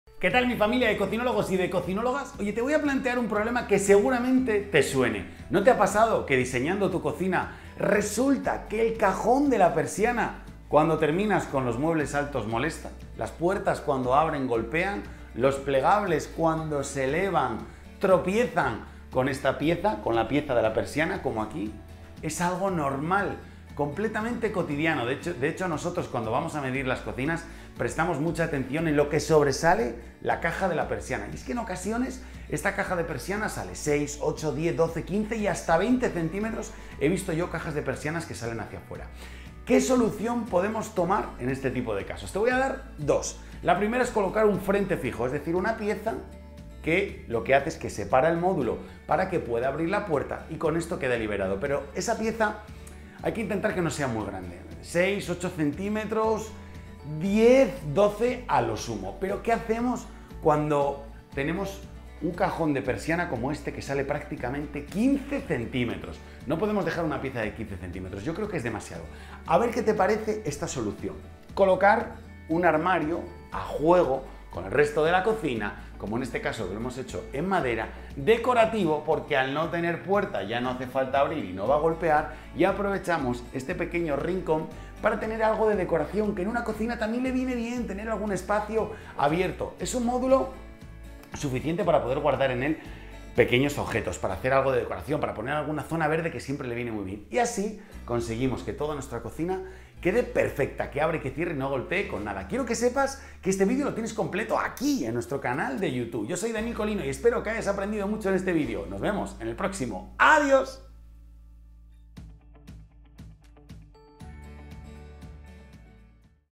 Muy ATENTO al cajon de la PERSIANA en la cocina CJR
⚠️ ¡CUIDADO! ⚠️ Ten en cuenta este detalle del cajón de la persiana de la cocina a la hora de hacer el diseño de cocina.

Si estás pensando el diseño de tu cocina y te coincide el cajón de la persiana donde los muebles altos de cocina, debes tener en cuenta un detalle. Y es que el cajon de la persiana en la cocina sobresale algunos centímetros. Lo cual supone un problema para abrir los muebles altos de cocina como en este caso con estos muebles plegables altos. ¿Qué se puede hacer? Pues se puede diseñar un frente fijo de cocina para evitar que el mueble alto de cocina golpee el cajon de la persiana o también existe la opción de diseñar un mueble decorativo de cocina que cumpla la misma función.

¿Qué os para la solución que hemos hecho en esta cocina cashmere en dos frentes?